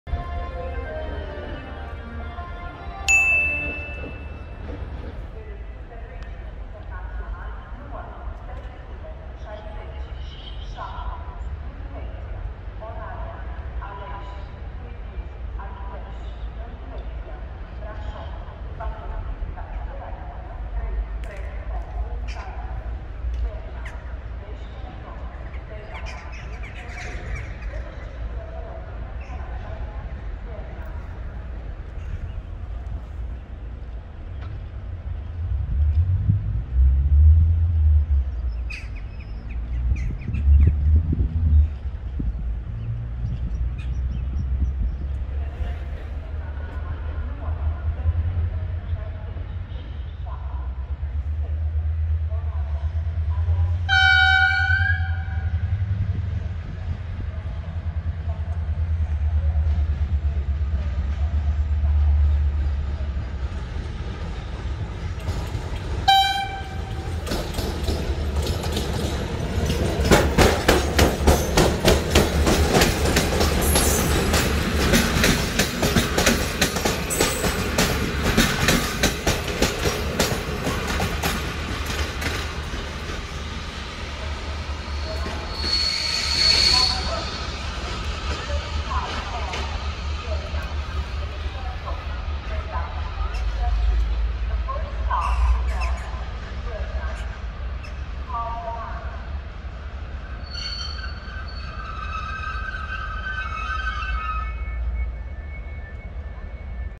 IR 367 "Harghita" Oradea - Brasov soseste in statia Cluj Napoca la linia 3
00:00 - 00:05 intro
00:05 - 00:31 tabela + anunt
00:31 - 01:09 IR 367 soseste in statia Cluj Napoca
01:09 - 01:12 Electroputere + General Motors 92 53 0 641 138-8
01:12 - 01:16 Remarul 16 Februarie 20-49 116
01:16 - 01:19 Remarul 16 Februarie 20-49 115
01:19 - 01:47 garare la linia 3

Total parcurs:
535km parcursi in 10 ore si 4 minute

Compunere tren:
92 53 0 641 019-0 Electroputere + General Motors EMD 2100HP 100kmh
1 x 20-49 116 : construit Astra Vagoane Arad, modernizat Remarul 16 Februarie: clasa 2
1 x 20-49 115 : construit Astra Vagoane Arad, modernizat Remarul 16 Februarie: clasa 2